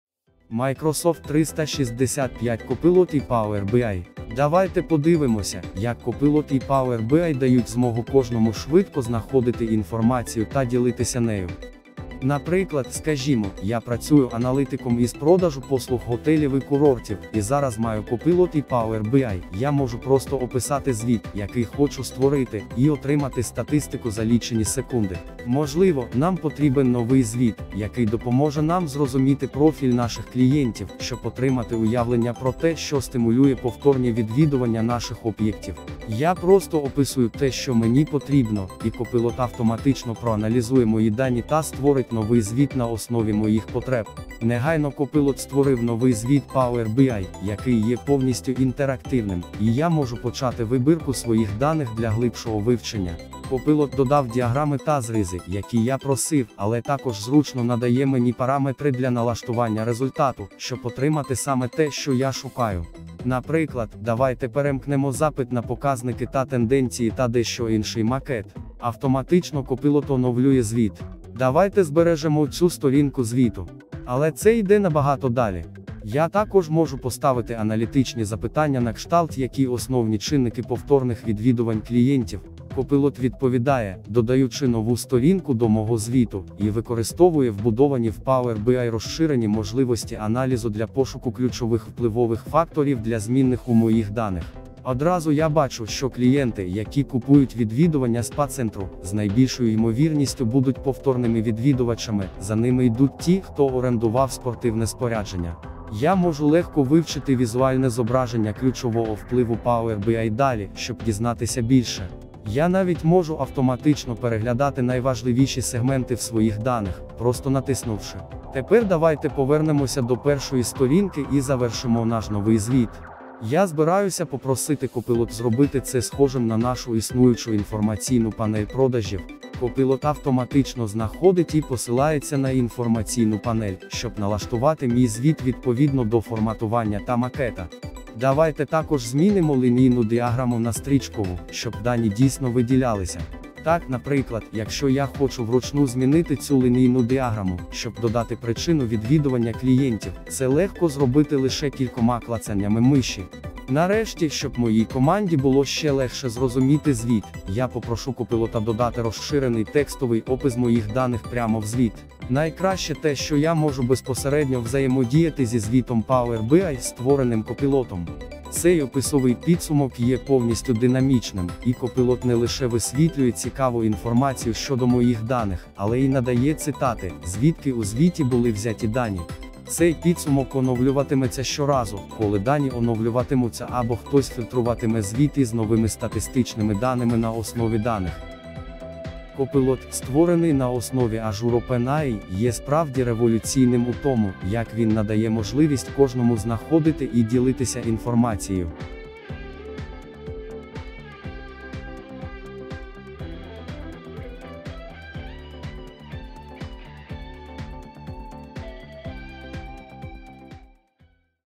✨Microsoft 365 Copilot and Power BI надає аналіз даних ще ефективніше
За допомогою Copilot у Power BI ми впроваджуємо потужність великих мовних моделей у Power BI на кожному рівні, щоб допомогти користувачам зробити більше та отримати більше цінності від своїх даних. Використовуючи Copilot, ви можете просто описати візуальні елементи та ідеї, які ви шукаєте, а Copilot зробить все інше. Користувачі можуть створювати та налаштовувати звіти за лічені секунди, генерувати та редагувати обчислення DAX, створювати описові підсумки та ставити запитання щодо своїх даних, і все це розмовною мовою. Завдяки можливості легко налаштовувати тон, обсяг і стиль оповідань і безперешкодно додавати їх у звіти, Power BI також може надавати аналіз даних ще ефективніше за допомогою простих для розуміння текстових підсумків.